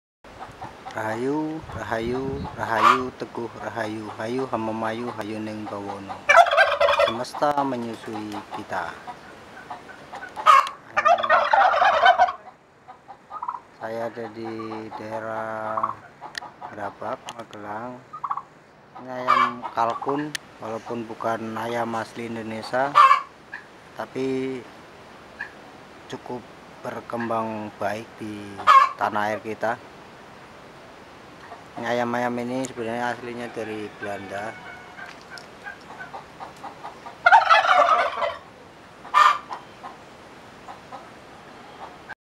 Ayam-ayam Kalkun Milik Pak Dul Kiwik Berkembang Baik di Grabag,Magelang
Meskipun ayamkalkun bukan asli Indonesia tapi ayam ini berkembang baik dan akrab dengan masyarakat Indonesia.